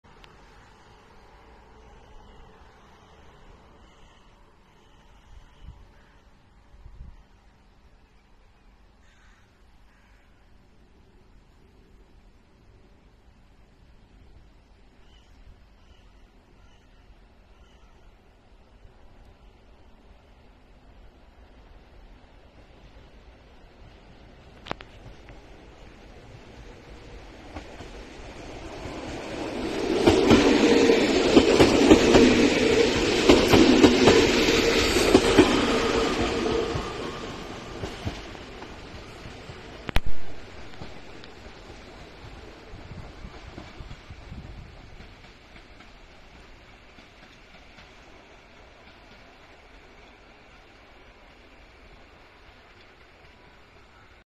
EN57-781
film nagrany 24 Września 2023r
relacja:Tarnów-Muszyna